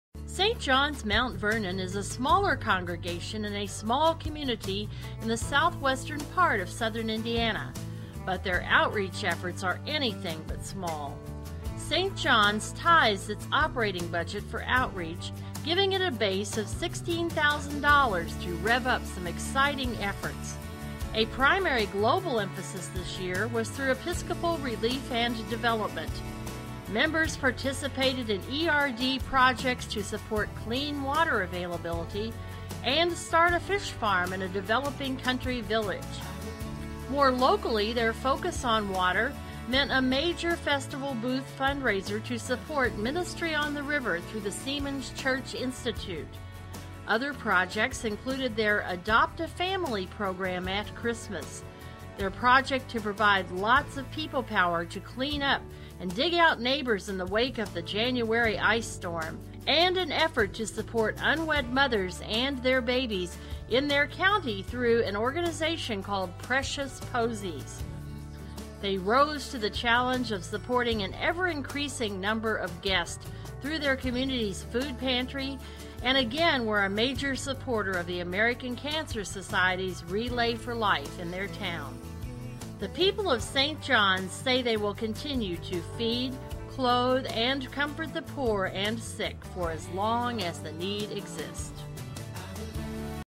St. John's North Vernon
St. John's Episcopal Church in North Vernon, Indiana  Mission Minutes from the Dioces of Indianapolis 2009 Convention